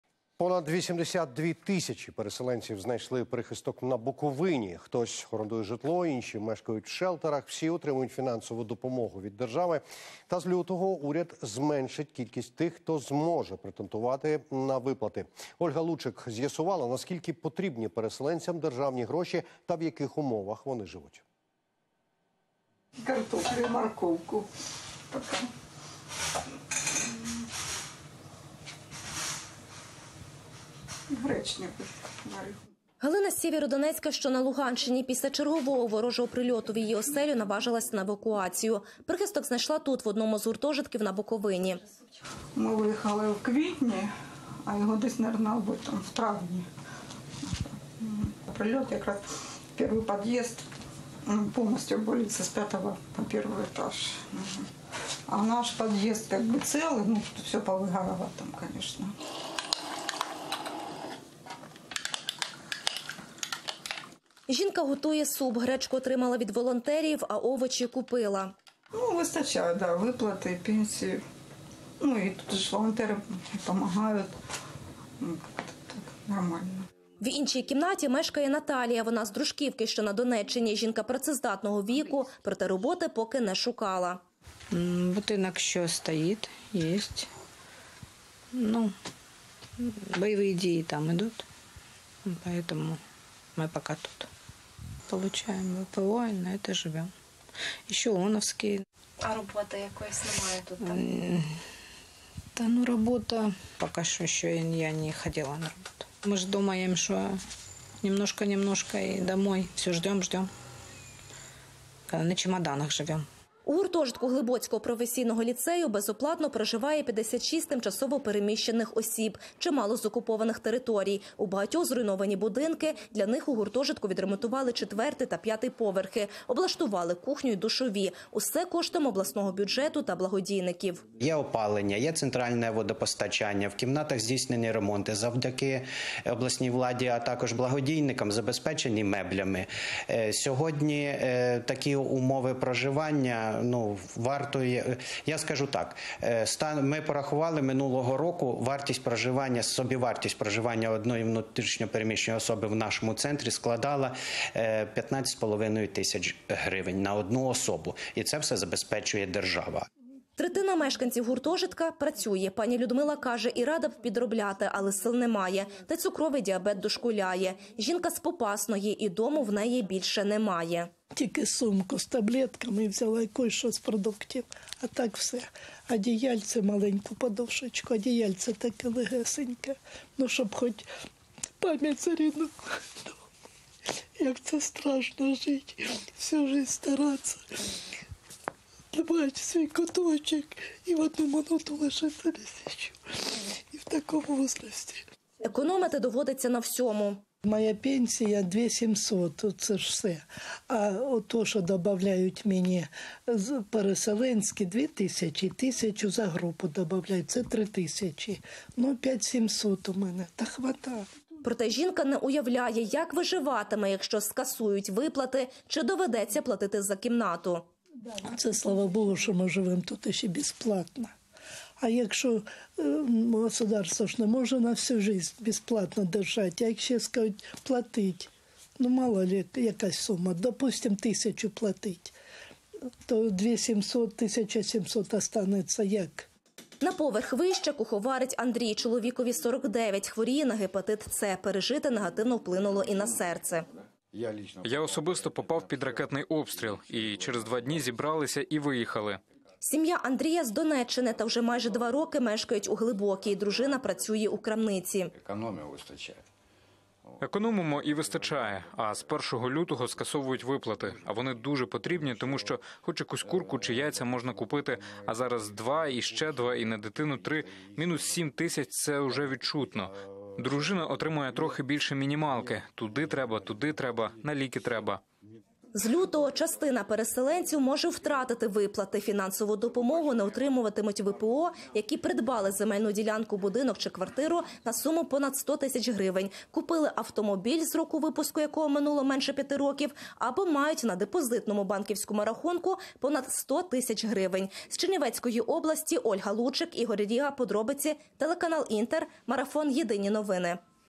Кількість отримувачів виплат ВПО зменшиться: реакція переселенців
З лютого уряд зменшить кількість тих, хто може претендувати на виплати. Ольга Лучек з'ясувала, наскільки потрібні переселенцям державні гроші та в яких умовах вони живуть.